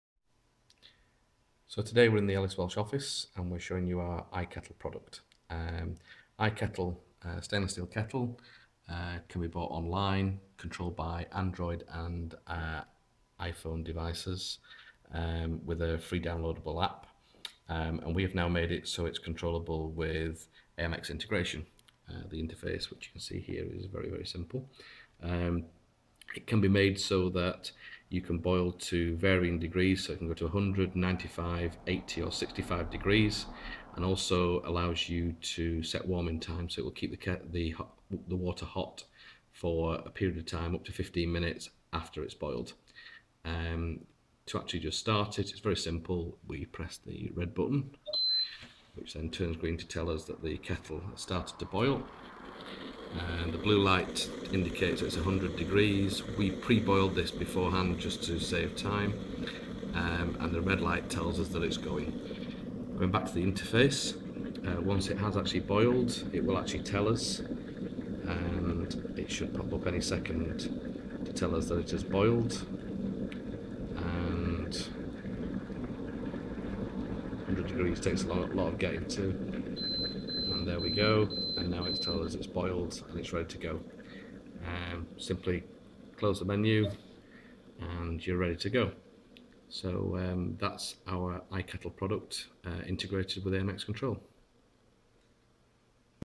kettle movie
The first AMX controlled WIFI Kettle. Available at Ellis Welch Portugal. Ours is now integrated with Kaleidescape,so intermission asks if you want to make a cuppa.